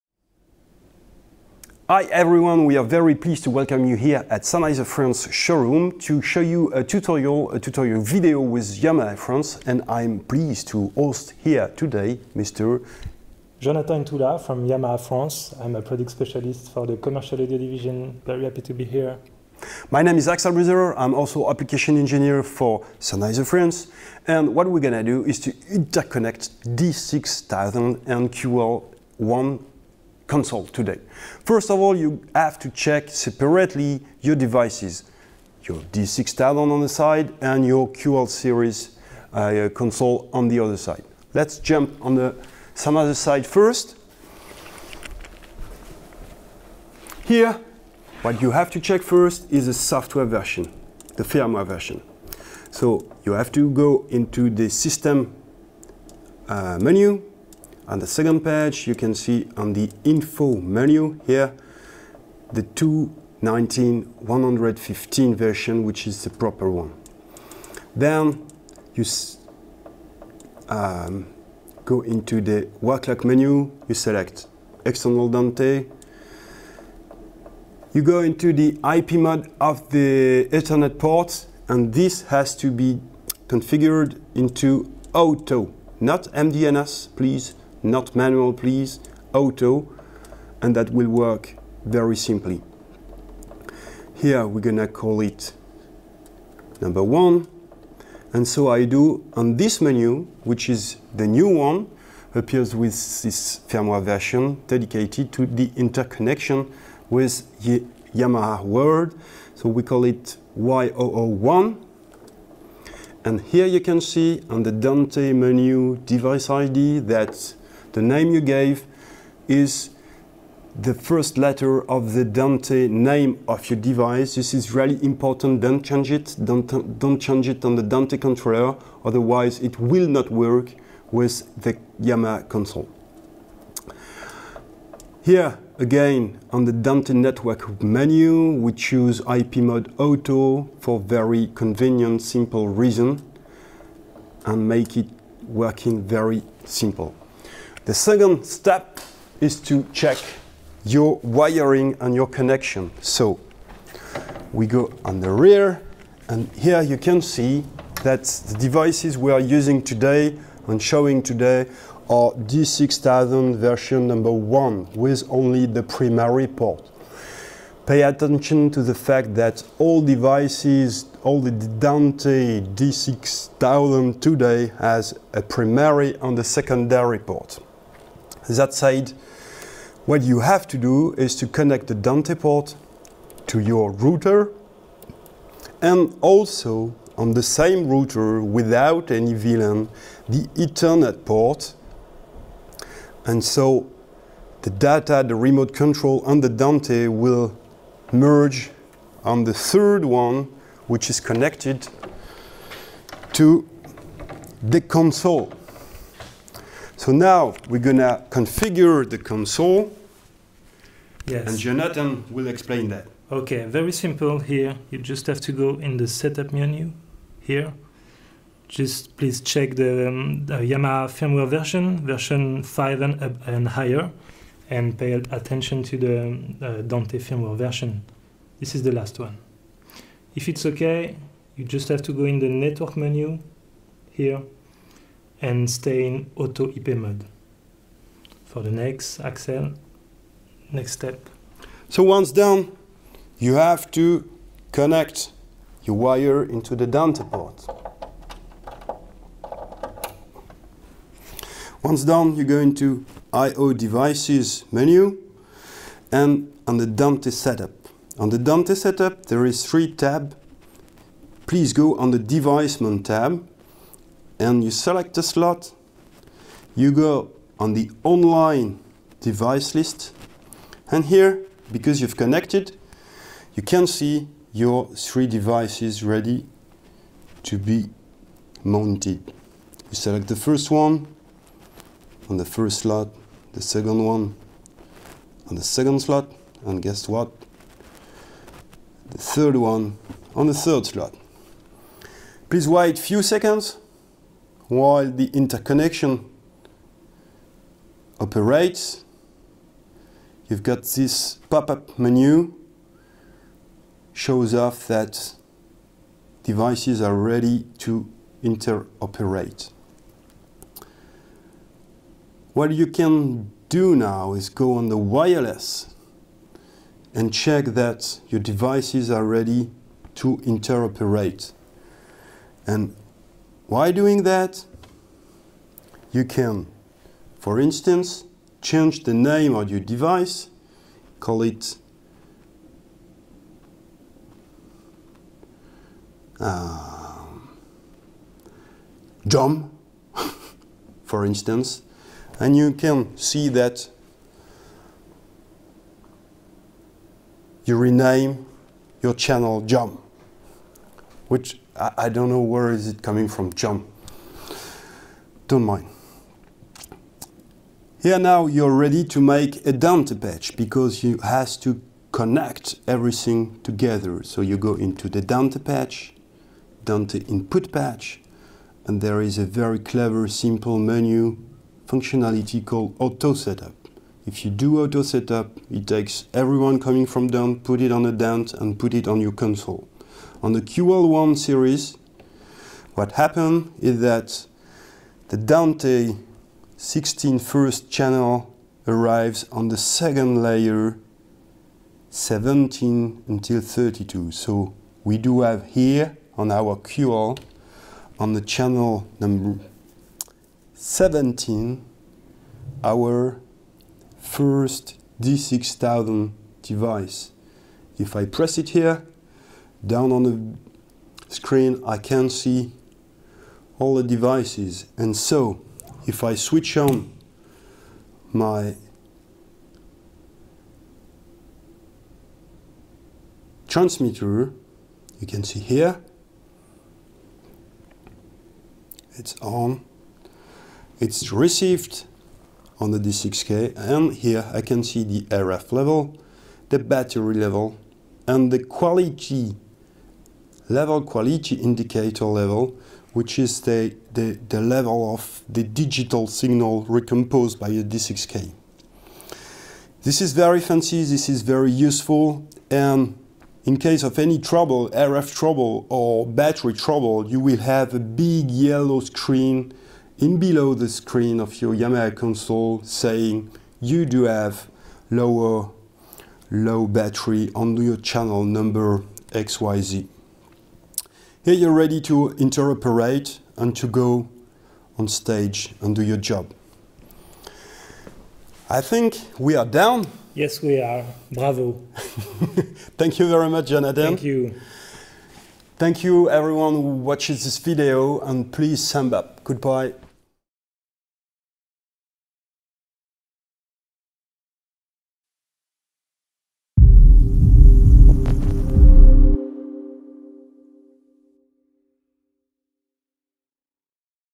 TUTO D6000 EN
Learn how to configure properly your Yamaha QL/CL series, Sennheiser EM6000 devices and network to display and remote control your receiver from the console touchscreen.

 1) Format your usb stick ON THE YAMAHA console
 2) copy to the root directory  the files
  a. MQLP5_01.PGM
  b. QL-MD103.BIN
  c. FWUpdate_QL1_v4082_sw206.dnt
 3) Switch off the console, remove all cables of it
 4) Insert the USB stick on the console, follow the update process.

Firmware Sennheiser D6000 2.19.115
Firmware Audinate Brooklyn II (Dante Type 1) v_2_0_3_11 (3.8 MB)

Firmware Audinate Brooklyn II (Dante Type 2) v_2_0_3_11 (3.8 MB)

Firmware EM 6000 v 2.1.9 (including SK/SKM 6000 v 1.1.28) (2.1 MB)